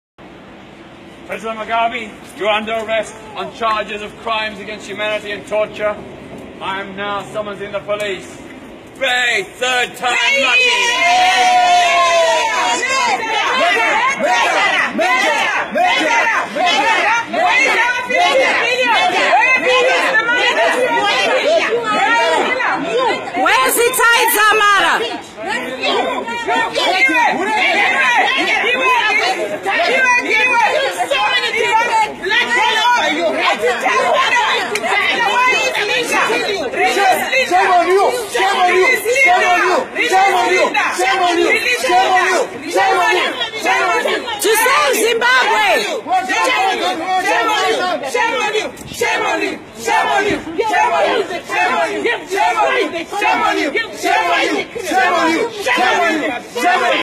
Peter Tatchell and Members of ROHR Zimbabwe and Zimbabwe Vigil catch Mugabe
Mugabe, Grace and John Mangudya are captured by activists and also Peter Tatchell, who has attempted twice beforte to arrest Mugabe for genocide. Recorded on 11th August 2016